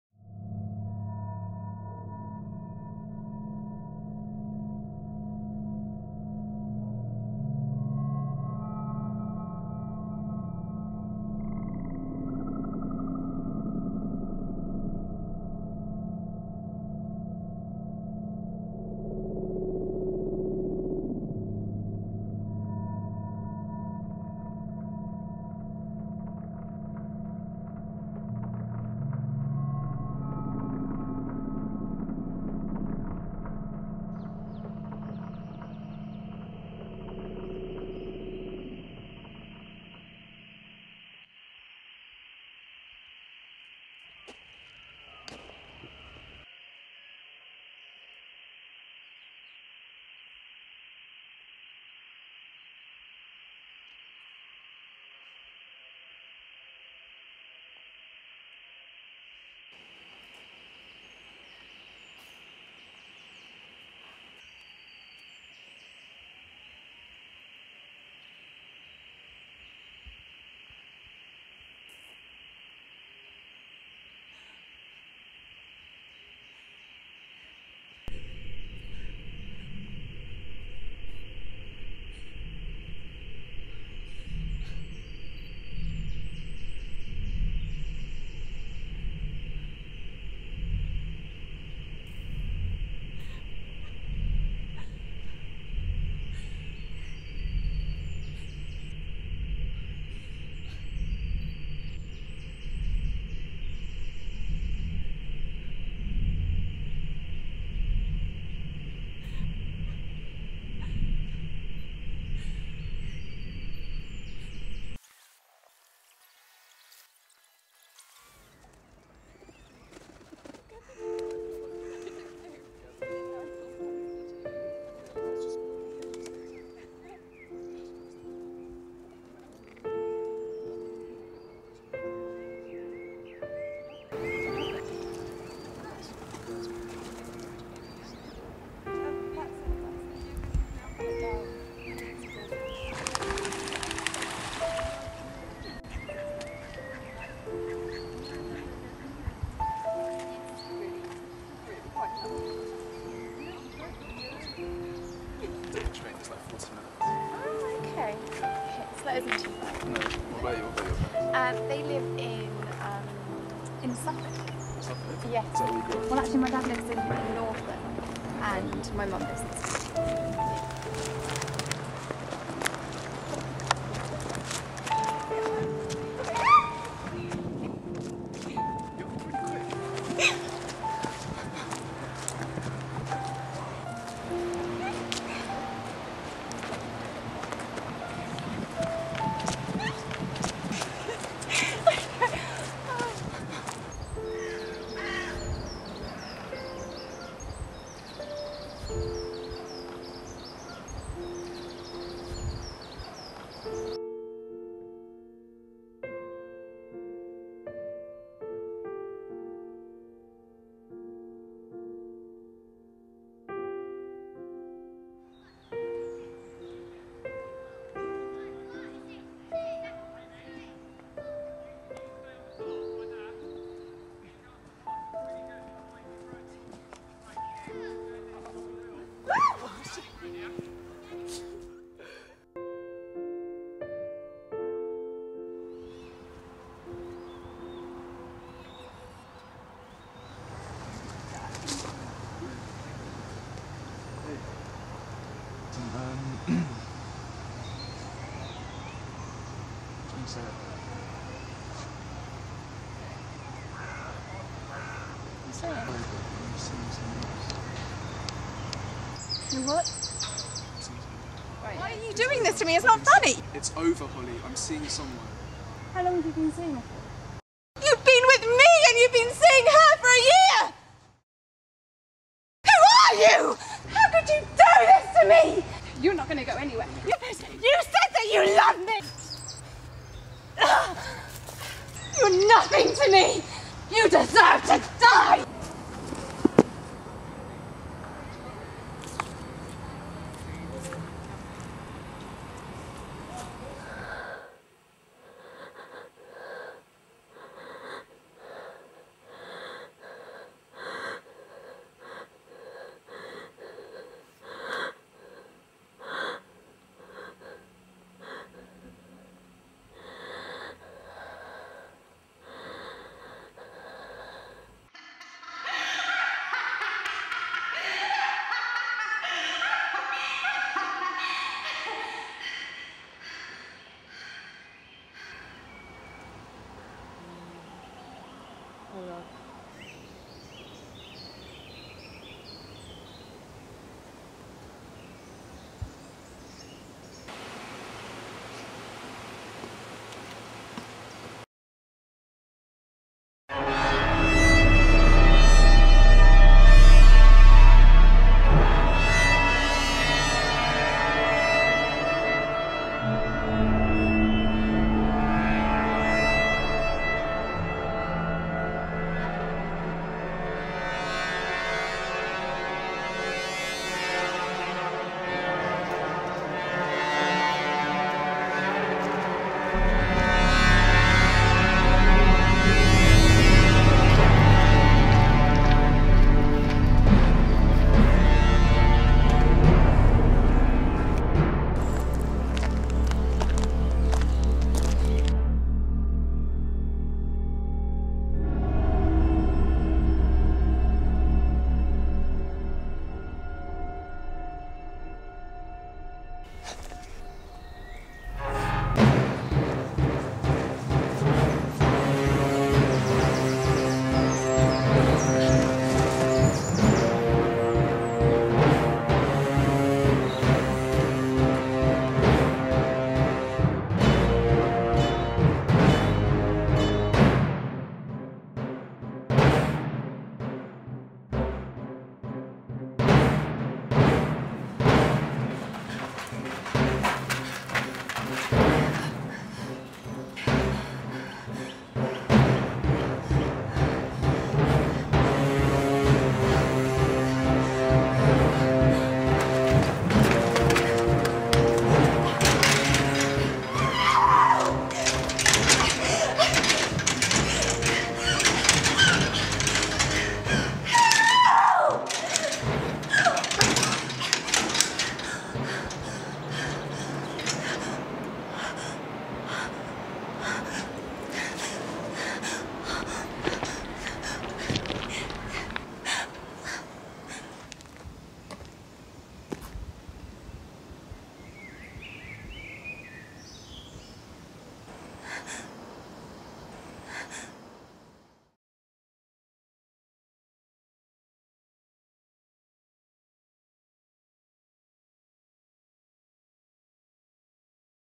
Hammer Horror: London College of Fashion BA (Hons) Fashion Illustration 2009
A short film based on the Hammer Horror genre by students on the BA (Hons) Fashion Illustration degree course a the London College of Fashion, University of the Arts London, 2009.